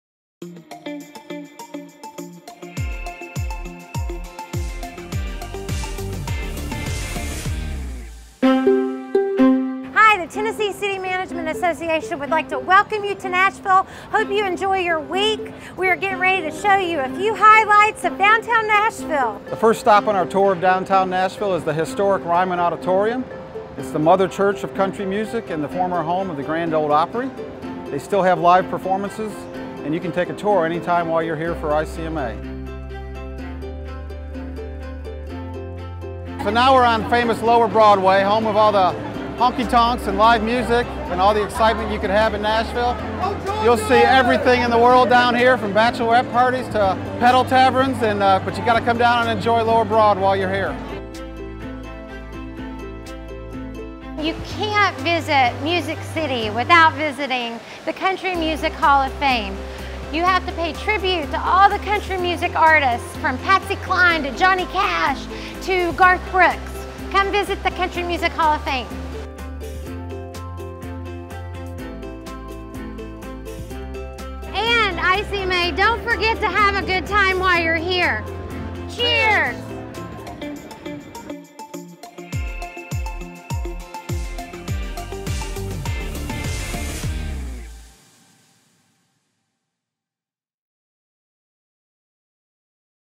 Nashville City Tour - ICMA 2019
The ICMA Host-Committee co-chairs take ICMA TV on a tour of Nashville. During the trip, ICMA visits Lower Broadway street, the famous Ryman Auditorium, and the Country Music Hall of Fame.

Find out more about ICMA, and the profession, at our